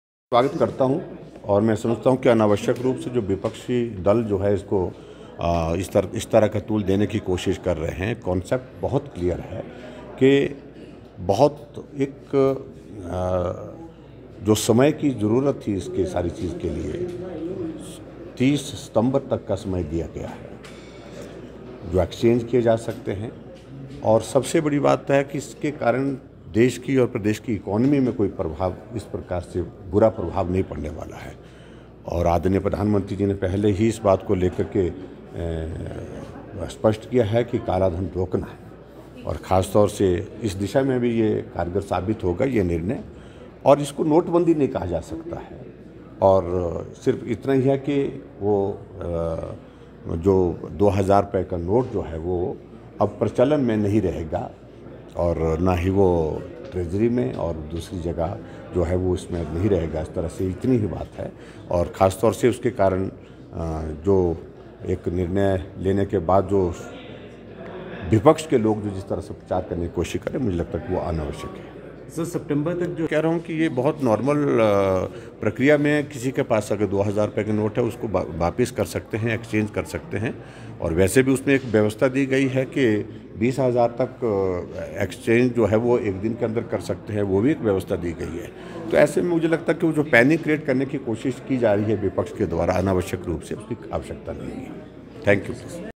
दो हजार का नोट बंद करना समय की आवश्यकता- जयराम ठाकुर || Himachal Dialogues
बेवजह पैनिक क्रिएट कर रहा विपक्ष, दो हजार का नोट बंद करना समय की आवश्यकता- जयराम ठाकुर

भारतीय रिजर्व बैंक ने दो हजार रुपए का नोट सरकुलेशन से बंद करने का फैसला लिया है. इसके लिए आम जनता को 30 सितंबर तक का वक्त दिया गया है. इस कदम में बाद विपक्ष सरकार पर हमलावर नजर आ रहा है. इस बीच हिमाचल प्रदेश के पूर्व मुख्यमंत्री और नेता प्रतिपक्ष जयराम ठाकुर ने विपक्ष पर पलटवार किया है. जयराम ठाकुर ने कहा कि विपक्ष बेवजह पैनिक क्रिएट करने की कोशिश कर रहा है. उन्होंने कहा कि दो हजार का नोट बंद करना समय की आवश्यकता है. इससे देश की अर्थव्यवस्था पर कोई नकारात्मक असर नहीं होगा.

पूर्व मुख्यमंत्री जयराम ठाकुर ने कहा कि भारतीय रिजर्व बैंक ने आम जनता को नोट बदलवाने और अपने नोटों को खाते में जमा करवाने के लिए पर्याप्त समय दिया है. ऐसे में किसी को घबराने की जरूरत नहीं है. विपक्ष के कुछ नेता लोगों में डर पैदा करने की कोशिश कर रहे हैं, लेकिन इससे कोई फायदा नहीं होगा. उन्होंने कहा कि आम जनता 30 सितंबर तक सुविधा अनुसार दो हजार रुपए के नोट या तो बदलवा सकती है या अपने खाते में जमा कर सकती है.